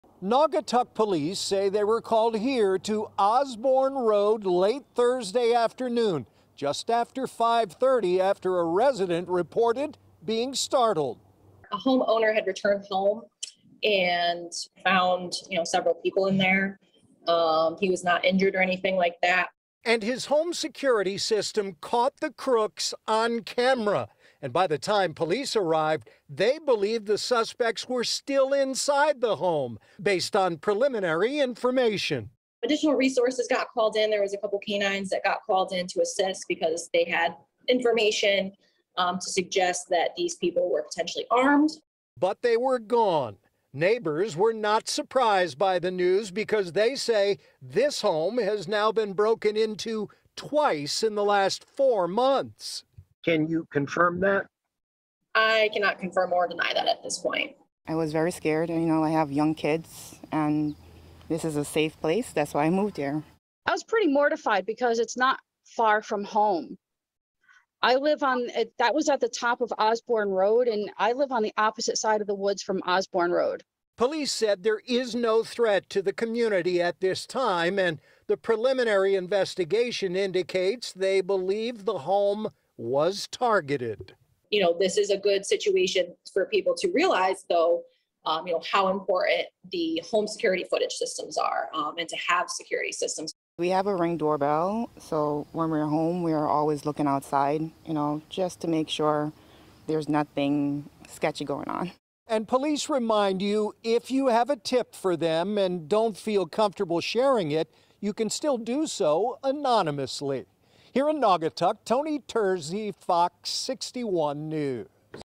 Naugatuck homeowners arrive during burglary
A video was uploaded to Facebook by the Naugatuck Police Department revealing footage from the burglary.